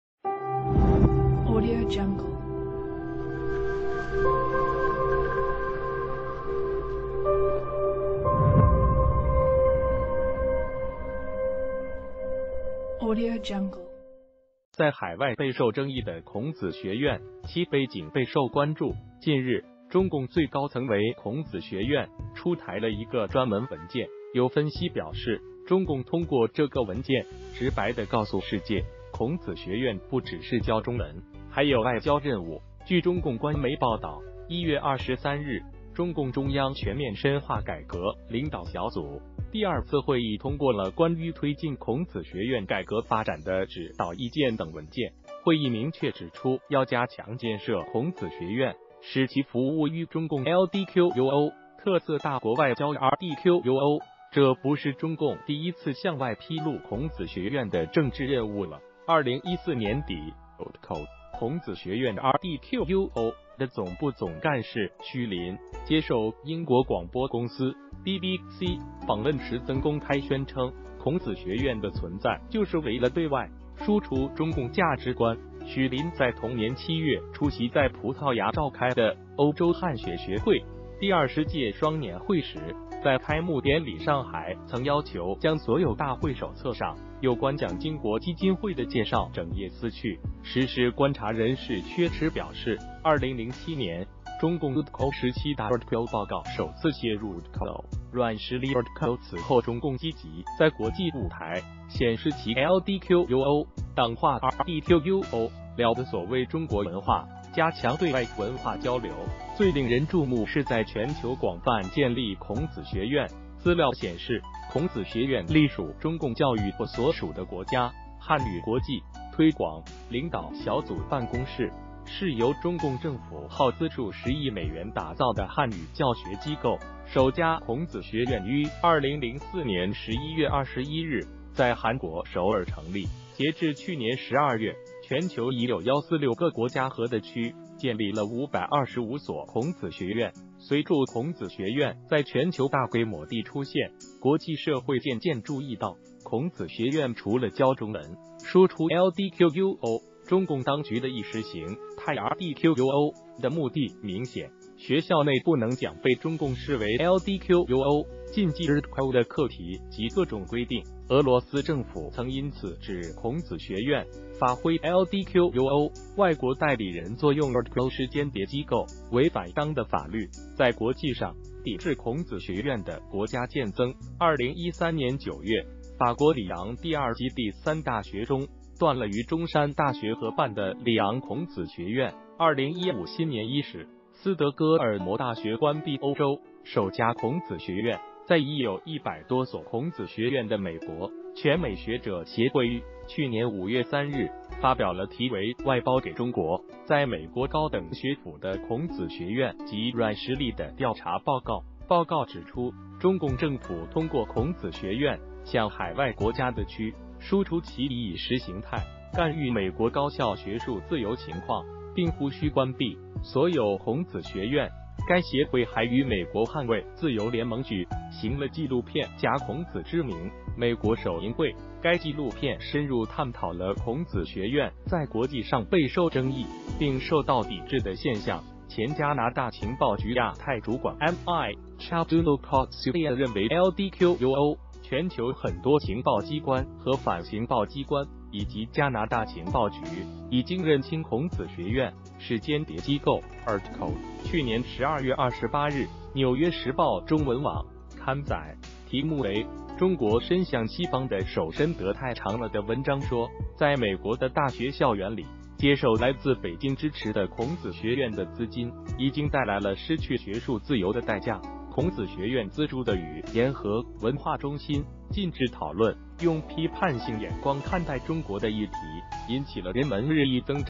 中共出台專門文件 孔子學院政治任務曝光
在海外備受爭議的孔子學院，其背景備受關注。近日，中共最高層為孔子學院出台了一個專門文件。有分析表示，中共通過這個文件直白地告訴世界，孔子學院不只是教中文，還有外交任務。據中共官媒報導，1月23日，中共中央全面深化改革領導小組第2次會議通過了《關於推進孔子學院改革發展的指導意見》等文件，會議明確指出要加強建設孔子學院，使其服務於中共&ldquo;特色大國外交&rdquo;。這不是中共第一次向外披露孔子學院的政治任務了。2014年底，&ldquo;孔子學院&rdquo;的總部總幹事許琳接受英國廣播公司（BBC）訪問時曾公開宣稱，孔子學院的存在就是為了對外輸出中共價值觀。許琳在同年7月出席在葡萄牙召開的歐洲漢學學會第20屆雙年會時，在開幕典禮上還曾要求將所有大會手冊上有關蔣經國基金會的介紹整頁撕去。時事觀察人士薛弛表示，2007年，中共&ldquo;十七大&rdquo;報告首次寫入&ldquo;軟實力&rdquo;，此後中共積極在國際舞台顯示其&ldquo;黨化&rdquo;了的所謂中國文化，加強對外文化交流，最令人注目是在全球廣泛建立孔子學院。資料顯示，孔子學院隸屬中共教育部所屬的國家漢語國際推廣領導小組辦公室，是由中共政府耗資數十億美元打造的漢語教學機構。首家孔子學院於2004年11月21日在韓國首爾成立；截至去年12月，全球已有146個國家和地區建立了525所孔子學院。隨著孔子學院在全球大規模地出現，國際社會漸漸注意到，孔子學院除了教中文，輸出&ldquo;中共當局的意識形態&rdquo;的目的明顯。學校內不能講被中共視為&ldquo;禁忌&rdquo;的課題及各種規定。俄羅斯政府曾因此指孔子學院發揮&ldquo;外國代理人作用&rdquo;，是間諜機構，違反當地法律。在國際上，抵制孔子學院的國家漸增。2013年9月，法國里昂第二及第三大學中斷了與中山大學合辦的里昂孔子學院；2015新年伊始，斯德哥爾摩大學關閉歐洲首家孔子學院。在已有100多所孔子學院的美國，全美學者協會於去年5月3日發表了題為《外包給中國：在美國高等學府的孔子學院及軟實力》的調查報告。報告指出，中共政府通過孔子學院向海外國家地區，輸出其意識形態，干預美國高校學術自由情況，並呼籲關閉所有孔子學院。該協會還與美國捍衛自由聯盟舉行了紀錄片《假孔子之名》美國首映會。該紀錄片深入探討了孔子學院在國際上備受爭議，並受到抵制的現象。前加拿大情報局亞太主管MichelJuneau-Katsuya認為：&ldquo;全球很多情報機關和反情報機關以及加拿大情報局已經認清孔子學院是間諜機構。&rdquo;去年12月28日，《紐約時報》中文網刊載題目為《中國伸向西方的手伸得太長了》的文章說，在美國的大學校園裡，接受來自北京支持的孔子學院的資金，已經帶來了失去學術自由的代價：孔子學院資助的語言和文化中心禁止討論用批判性眼光看待中國的議題，引起了人們日益增長的擔憂。